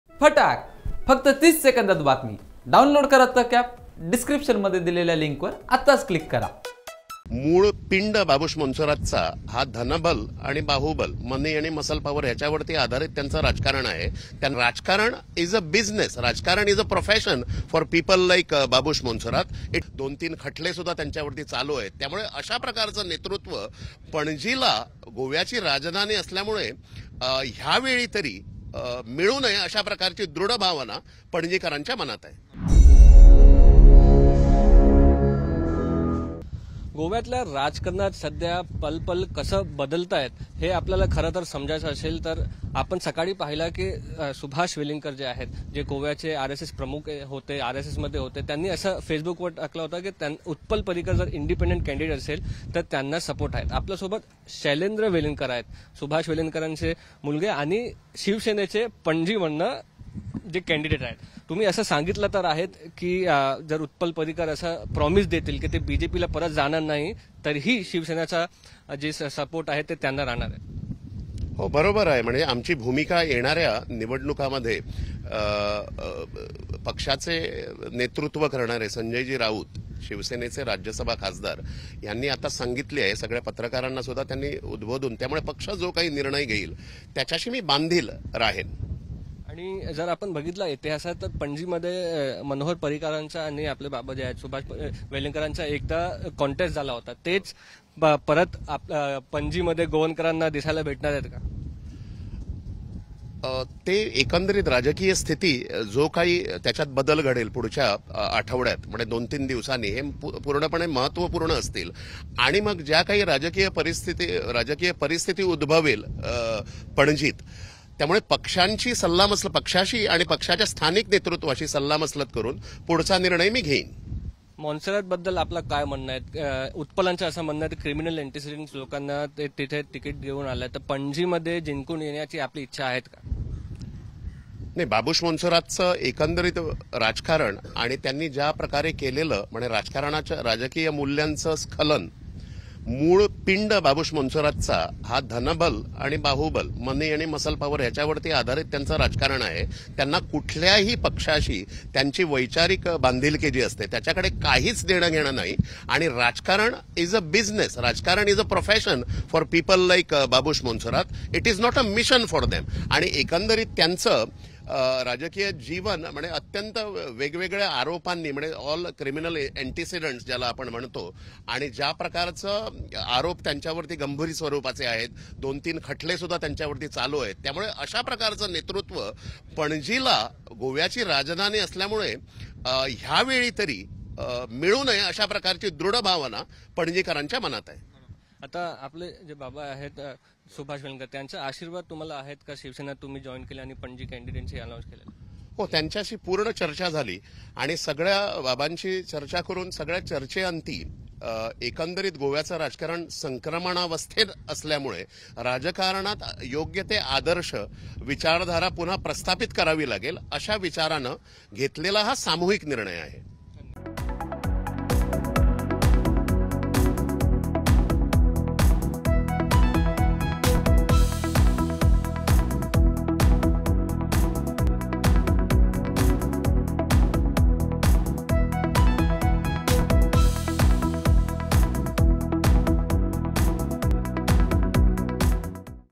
Goa Assembly Election 2022 : पर्रीकरांच्या पणजीत शिवसेनेची एंट्री | Utpal Parrikar | Velingkar
पर्रीकरांच्या पणजीत शिवसेनेची एंट्री, उत्पलला पाठिंबा आणि बाबूशच्या अडचणीत होणार का वाढ?